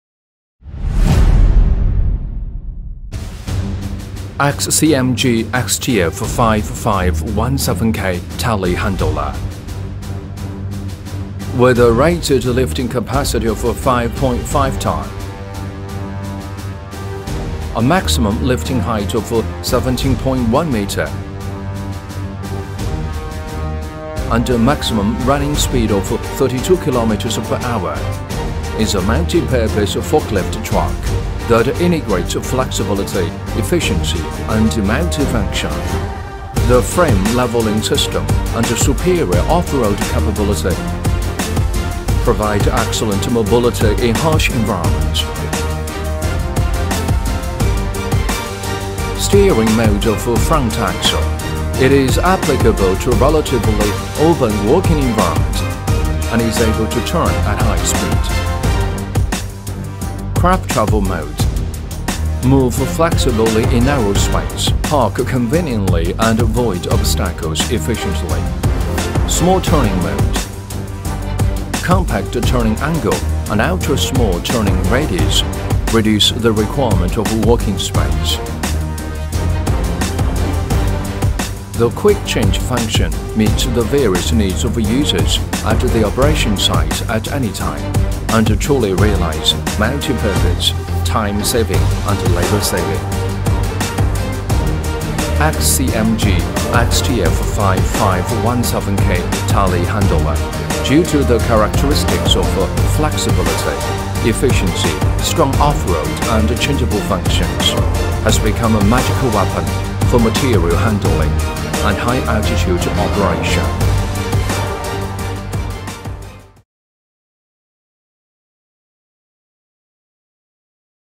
XCMG XTF5517K Telehandler—a Magical Weapon for Material Handling and High Altitude Operation
XCMG XTF5517K Telehandler,  with characteristics of flexibility, efficiency, strong off-road and changeable functions, which can work in steering mode of front axle, crab travel mode and small turning mode. In order to quickly change the functions to meet the various needs to users. Today  XCMG XTF5517K Telehandler has become a magical weapon for material handling and high altitude operation.